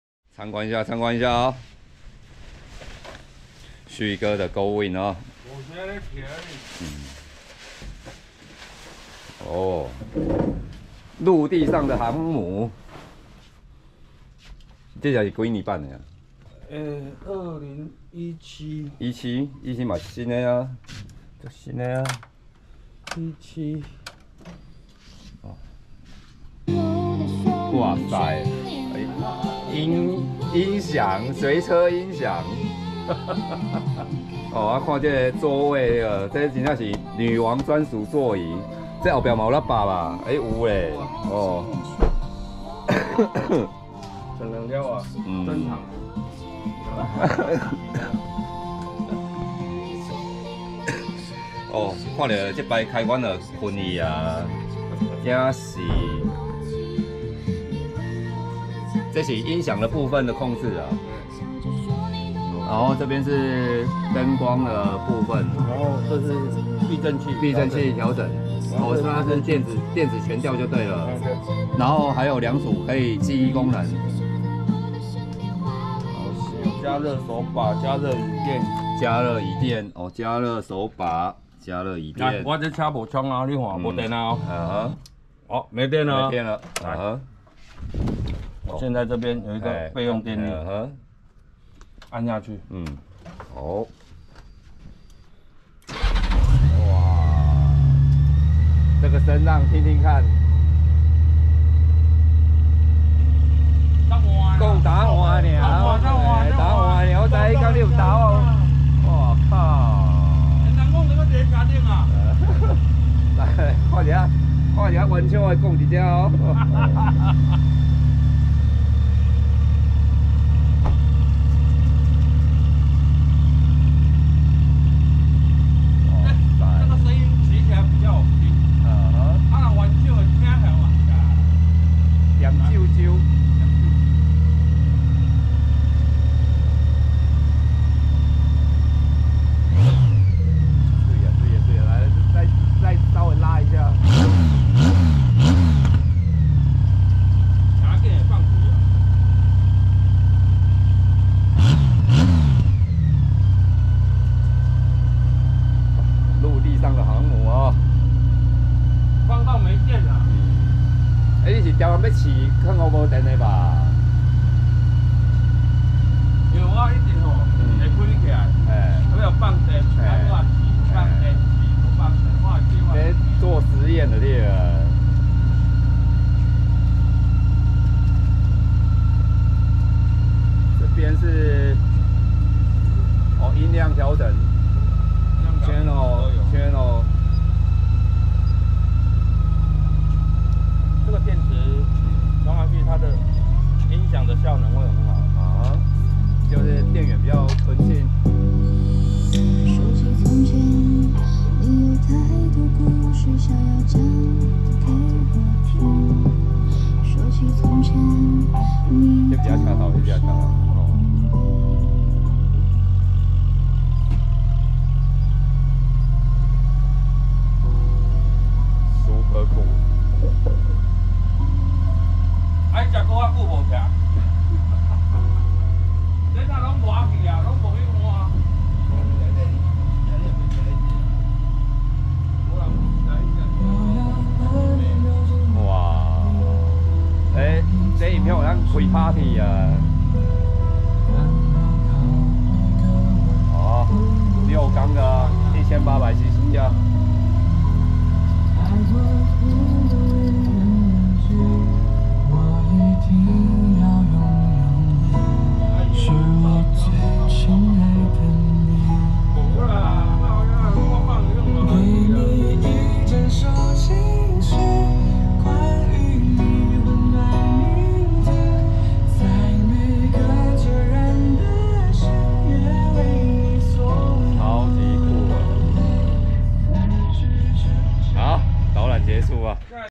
2019 12 18 2017 HONDA GOLDWING 賞車 VLOG
有幸可以參觀旭哥的愛駒.陸地上的航母.其豪華配備令人瞠目結舌.那音響真是讚啊